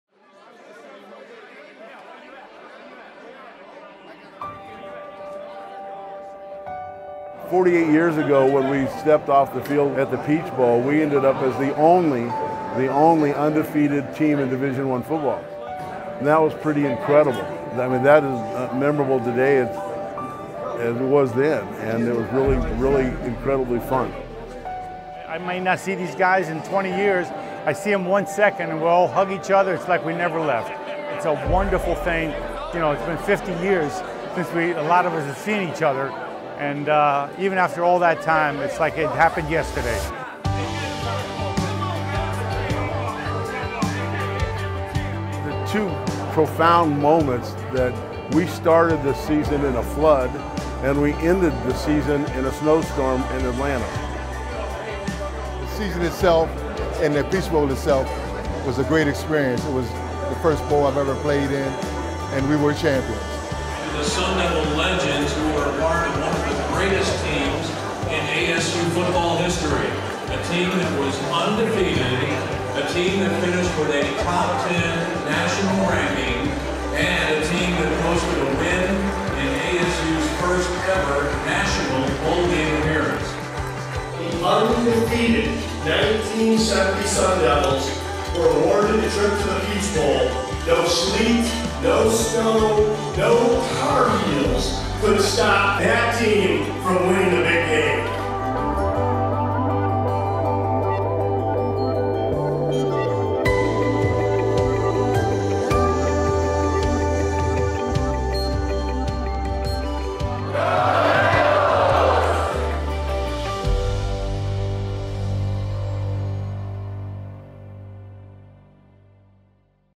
2018 ASU Alumni's Legends Luncheon
This annual Sun Devil tradition features giants in ASU football history. The 2018 Legends Luncheon honored the undefeated 1970 Peach Bowl champions, the team that earned Arizona State University its first-ever bowl victory!

J.D. Hill, Steve Holden, Jim Kelley, Steve Matlock, Mike Messenger, Joe Spagnola, Mike Tomco and Kevin Woudenberg are among the players who returned for the luncheon. Coaches taking part in the event include ends-linebackers coach Larry Kentera, receivers coach Joe McDonald, offensive line coach Al Tanara and defensive line coach Jerry Thompson.

The Peach Bowl victory became football head coach Frank Kush’s 100th victory at Arizona State.